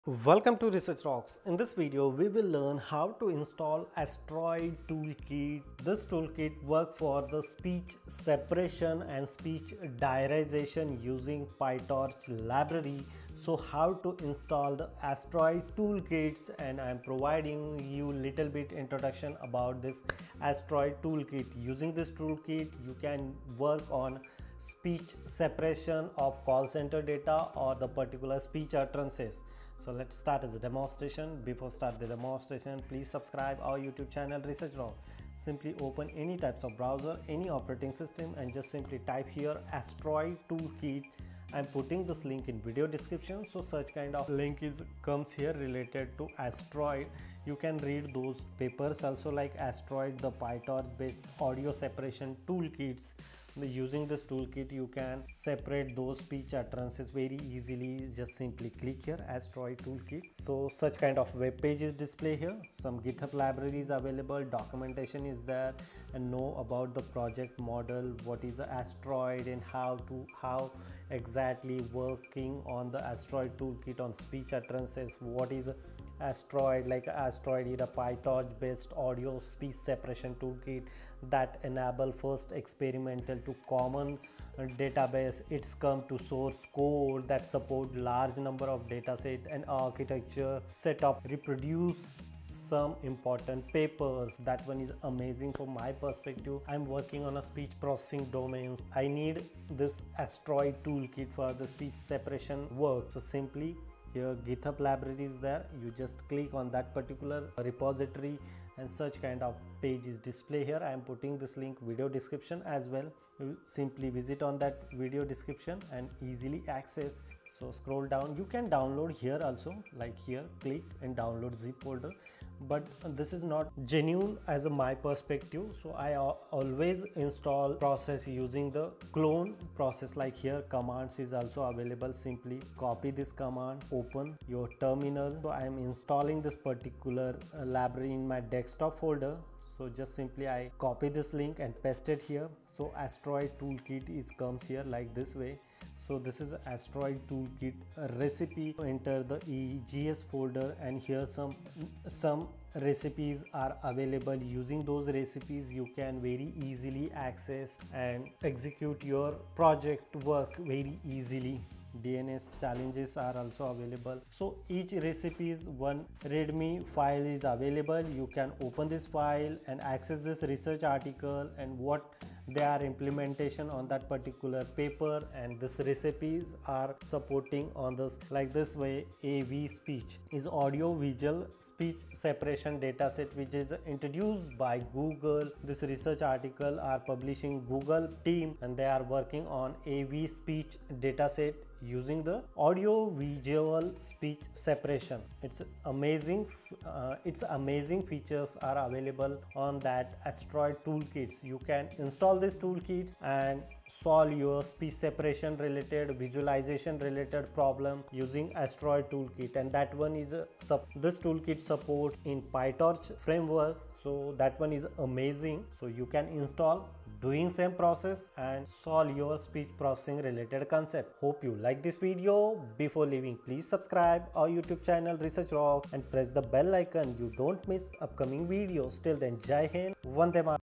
How to Install Asteroid Toolkit for Audio Source Separation with PyTorch
In this video we will see how to install Asteroid toolkit,  the PyTorch-based  audio source separation toolkit for researchers.  Inspired by the most successful neural source separation systems, it provides all neural building blocks required to build such a system. To improve reproducibility, Kaldi-style recipes on common  audio source separation datasets are also  provided.   This  paper  describes the software architecture of Asteroid and its most important features. By showing experimental results obtained with Asteroid’s recipes, we show that our implementations are at least on par with most results reported in reference papers.